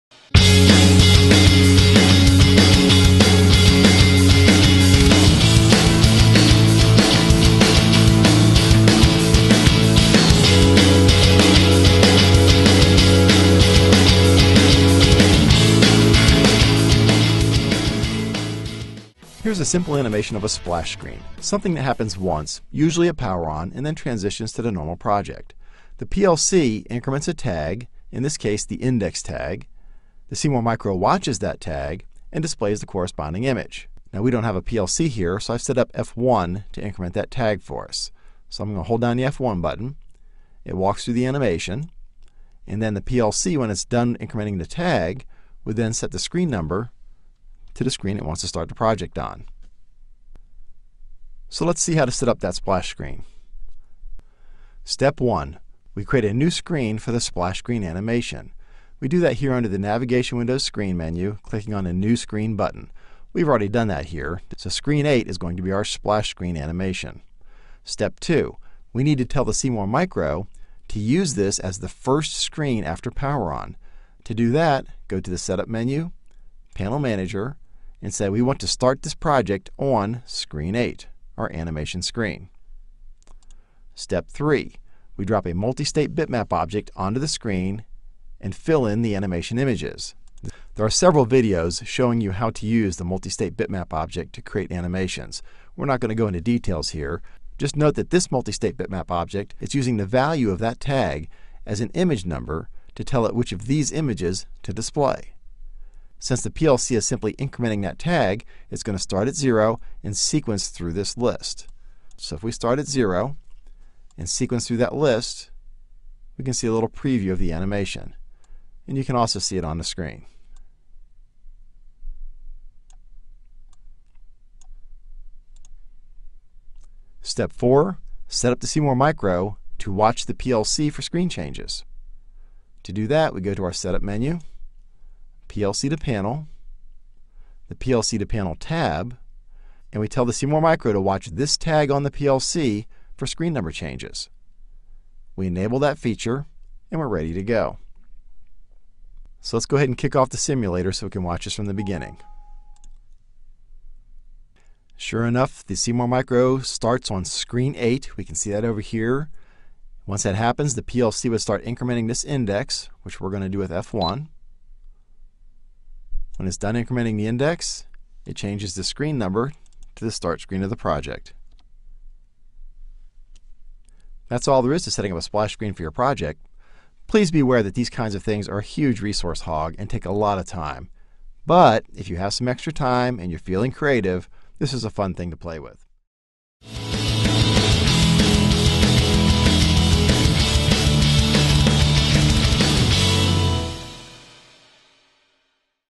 Animacion pantalla táctil PLCs
Realiza animaciones en la pantalla táctil para PLCs con tal solo 4 pasos rápidos y sencillos. Visualiza este video corto que te mostrará como hacerlo.